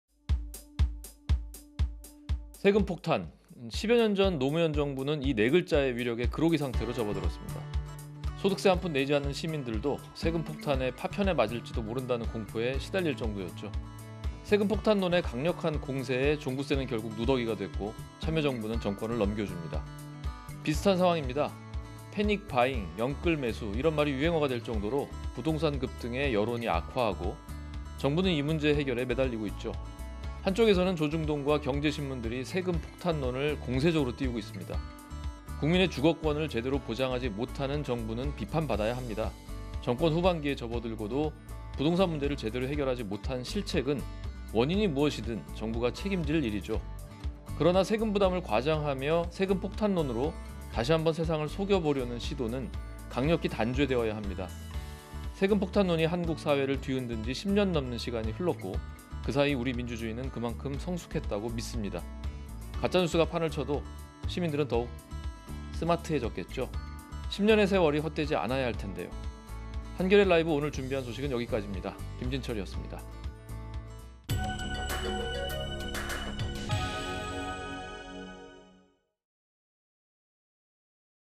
노무현 정부에 쏟아부은 세금폭탄론의 부활
세금폭탄.
10여년 전 노무현 정부는 이 네글자의 위력에 그로기 상태로 접어들었습니다.
소득세 한 푼 내지 않는 시민들도 '세금폭탄'의 파편에 맞을 거라는 공포에 시달릴 정도였죠.
세금폭탄론의 강력한 공세에 종부세는 결국 누더기가 되고 참여정부는 정권을 넘겨줍니다.

비슷한 상황입니다.
패닉바잉, 영끌매수 등 부동산 급등에 여론이 악화하고, 정부는 이 문제 해결에 매달리고 있죠. 한쪽에선 조중동과 경제신문들이 세금폭탄론을 공세적으로 띄우고 있습니다.

국민의 주거권을 제대로 보장하지 못하는 정부는 비판받아야 합니다. 정권 후반기에 접어들고도 부동산 문제를 제대로 해결하지 못한 실책은, 원인이 무엇이든, 정부가 책임질 일입니다. 그러나 세금부담을 과장하며 세금폭탄론으로 다시 한 번 세상을 속여보려는 시도는 강력히 단죄되어야 합니다.

세금폭탄론이 한국사회를 뒤흔든 지 10년 넘는 시간이 흘렀고, 그 사이 우리 민주주의는 그만큼 성숙했다고 믿습니다. 가짜뉴스가 판을 쳐도, 시민들은 더욱 스마트해졌겠죠. 10년의 세월이 헛되지 않아야 할 텐데요.